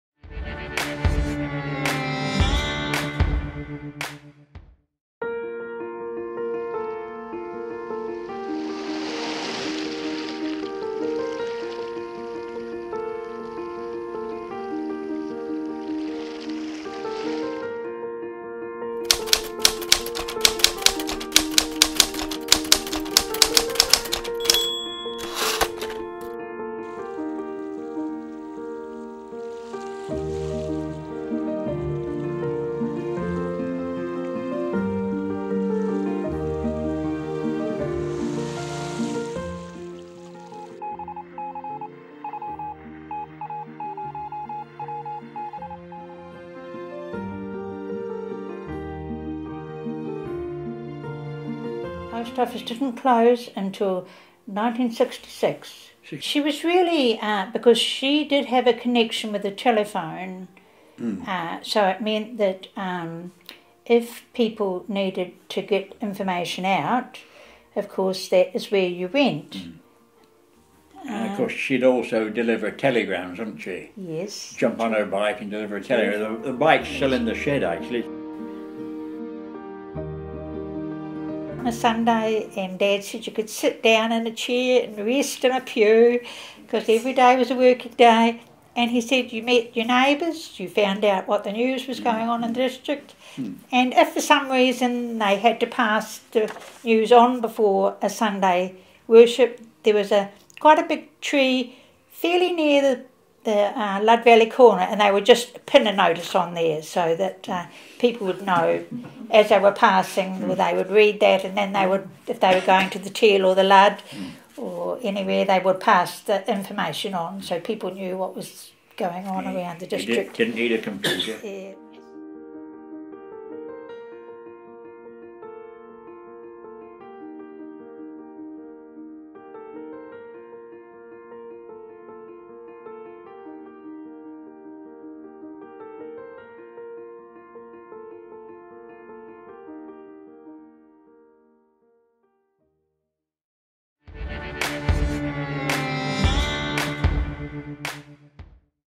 NVHF Bush Telegraph
Vicki Smith's story of early communication in the Wakapuaka area